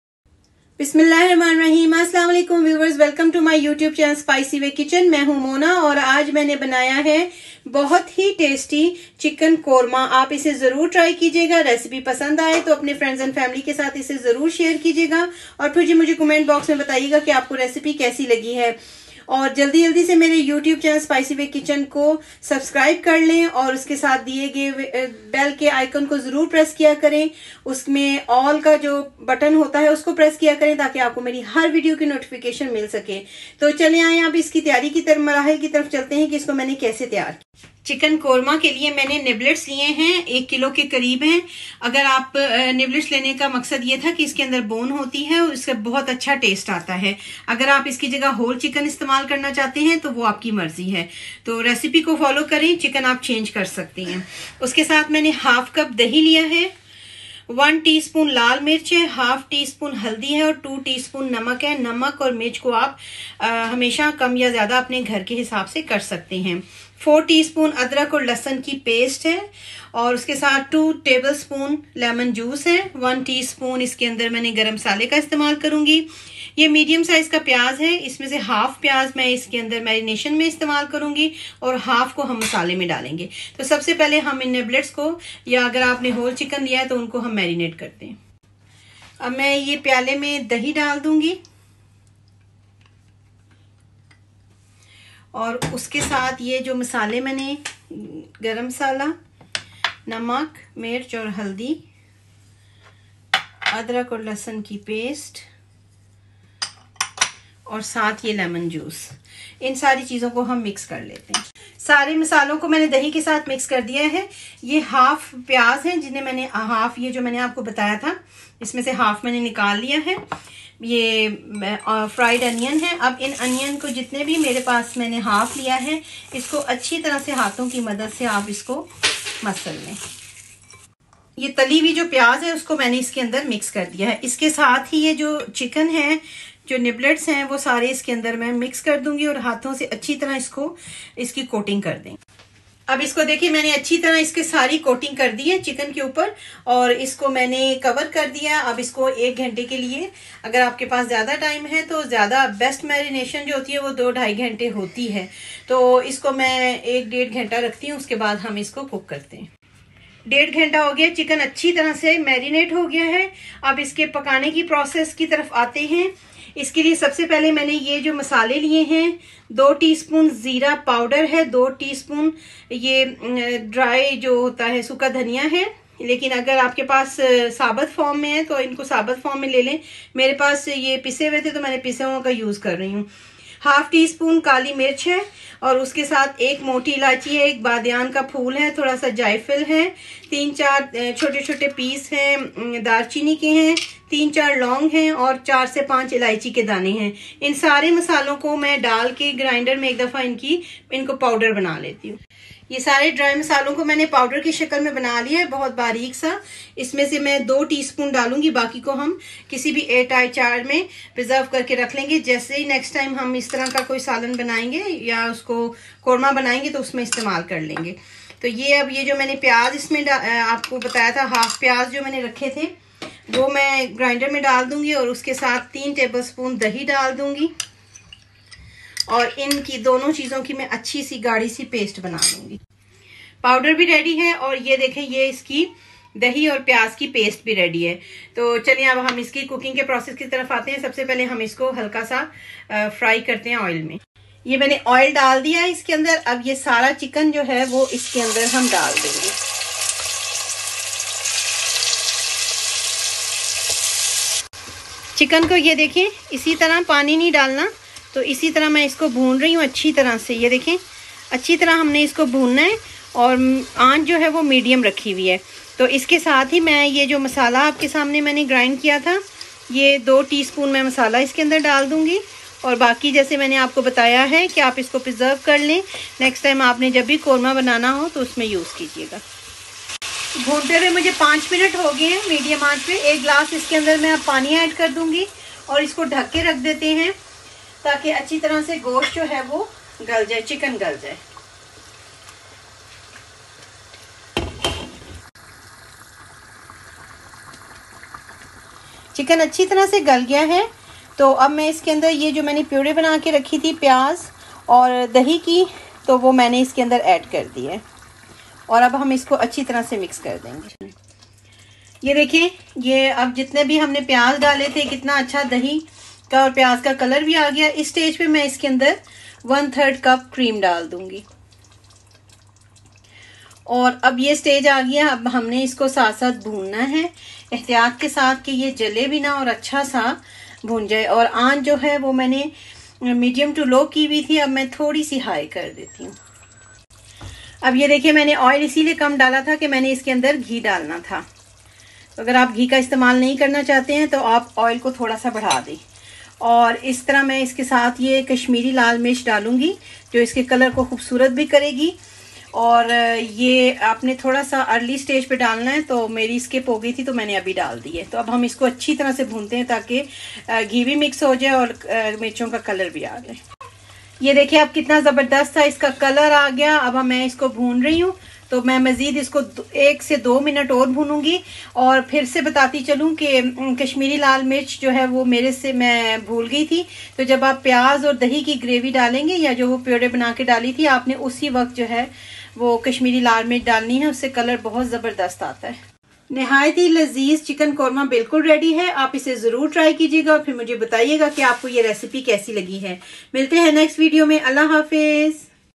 Chicken Korma -Chicken Korma Recipe - How to Make Chicken Korma -Shahi Chicken Korma Recipe-
Chicken Korma @Spicywaykitchen
Hi friends here is a tasty n yummy recipe of chicken korma with a aroma of home made special chicken korma powder to tasty n easy you make this recipe of chicken korma hope you will like n share this recipe with your friends n family  thanks

INGREDIENTS

Fried onion                       1 medium

Special Korma powder 🌟

Dry coriander                   2 tsp
Black pepper powder     1:2 tsp
Black cardamon              1
Star anis                            1
Jayfil                                   Small piece
Green elaichi                      3 to 4

Kashmiri red chilli             1 n half tsp